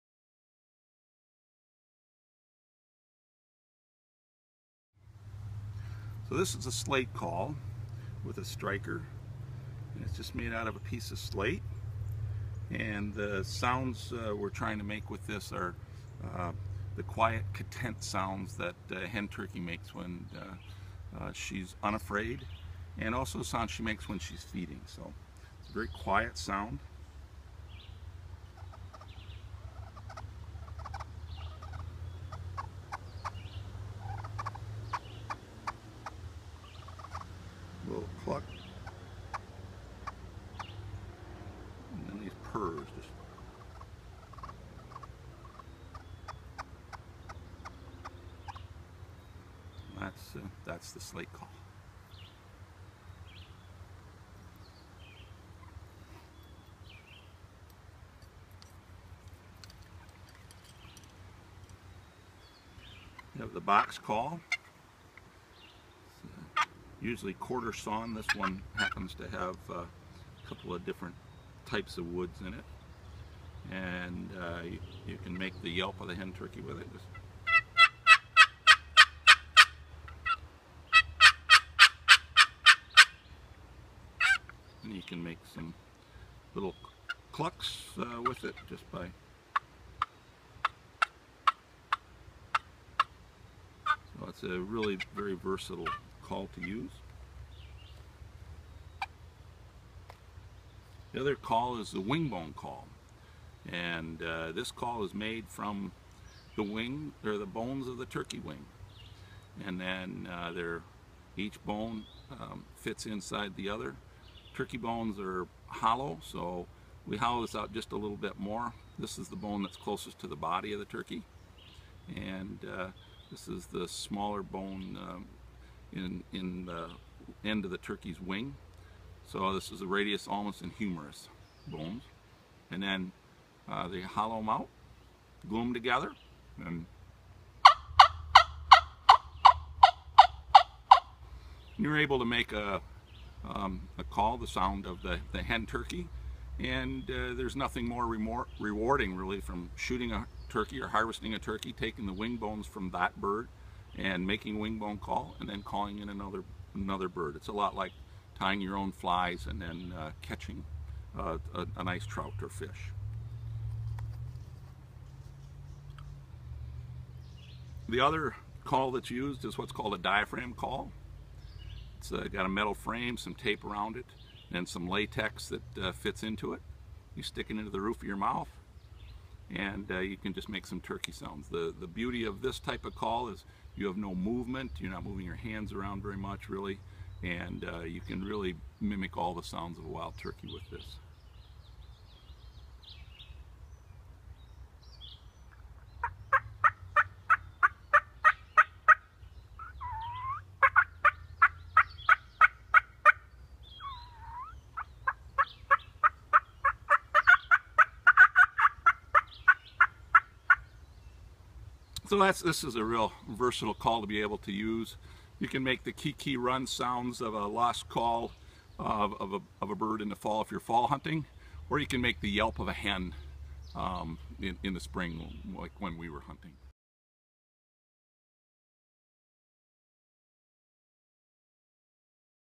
Turkey Calling
How to call in a turkey

A demonstration on how to use three different types of turkey call by Al Stewart, upland gamebird specialist at the Michigan Department of Natural Resources in the US.